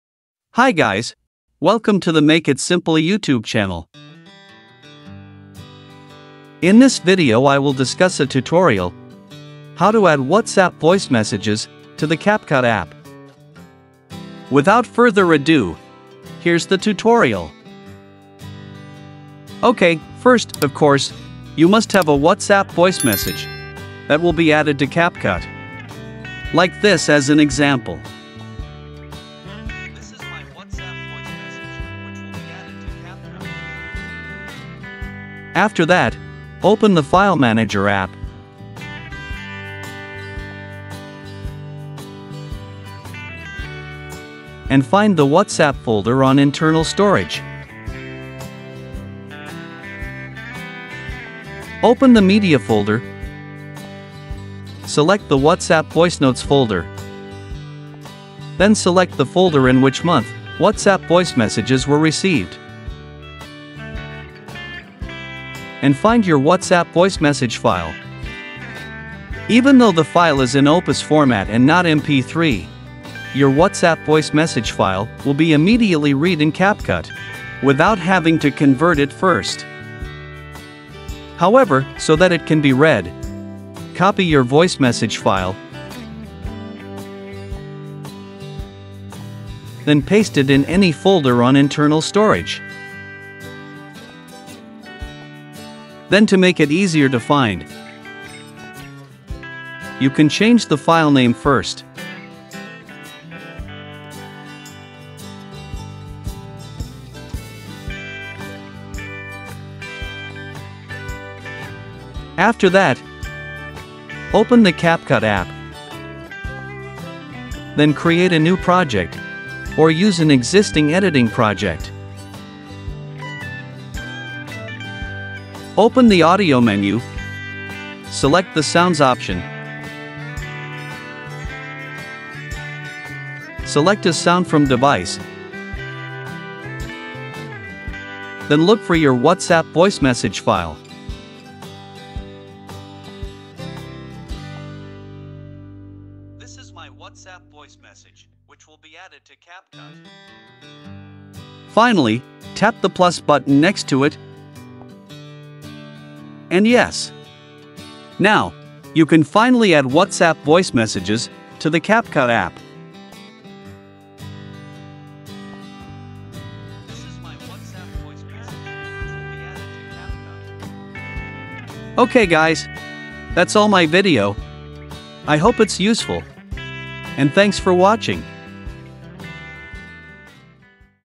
How to Add WhatsApp Voice Messages to Video with CapCut
How to add WhatsApp audio to CapCut. In this video, we will guide you step by step on how to add WhatsApp voice messages to your video using the CapCut app. This process involves finding the WhatsApp voice message file in your internal storage, copying it to another folder, and renaming it for easy access.

The first step is to open your phone's internal storage to locate the WhatsApp voice message file that you want to import into CapCut. Once found, copy the file to another folder for easier access. Don't forget to rename the file to make it more identifiable.

Next, open the CapCut app and create a new project or use an existing editing project. Navigate to the Audio menu and select the "Sounds" option. Choose audio from your device, find the WhatsApp voice message file you copied earlier, and tap the plus button to add it to the CapCut editing timeline.

By following this step-by-step guide, you'll easily sync your favorite WhatsApp voice messages with the video you want to create. Remember to save your project and share your finished video with the world!